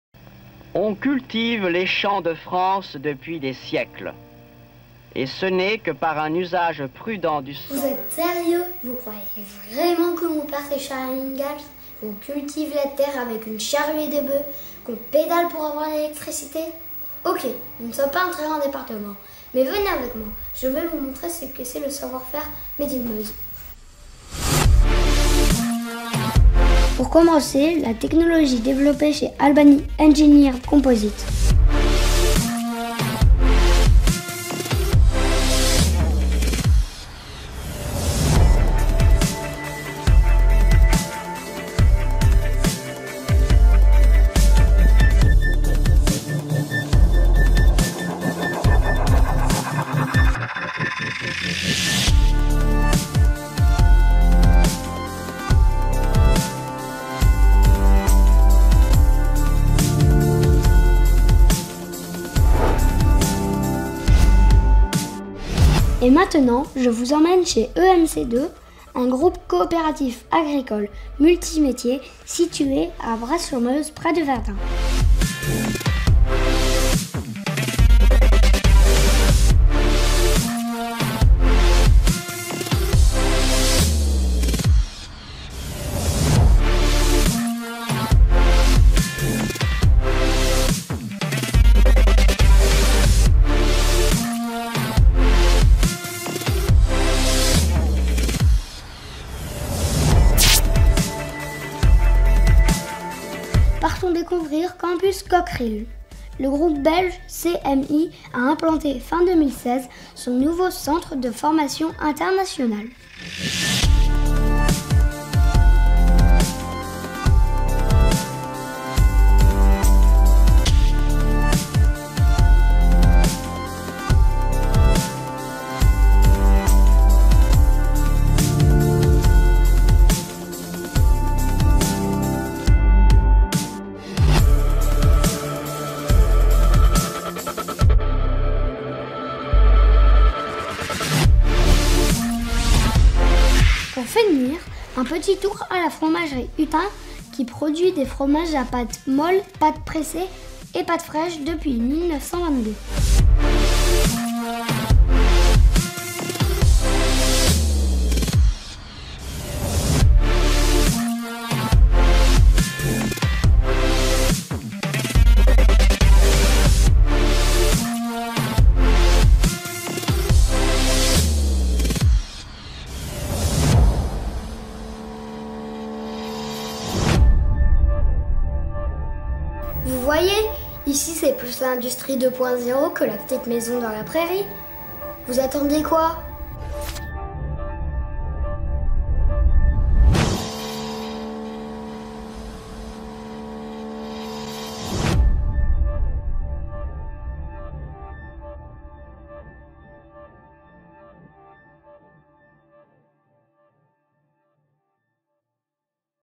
Découvrez le savoir-faire Made in Meuse !
Safran à Commercy, EMC2 à Bras sur Meuse, CMI Campus Cockerill à Commercy et la Fromagerie Hutin Hochland à Dieue sur Meuse